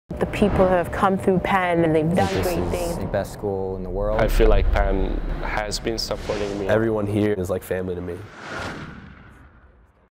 Images Of The Year 2016-17
Penn Athletics: 2-16-17 Images of the Year

Relive the championship wins, record-setting performances and memory-making moments from 2016-17 for the Penn Quakers.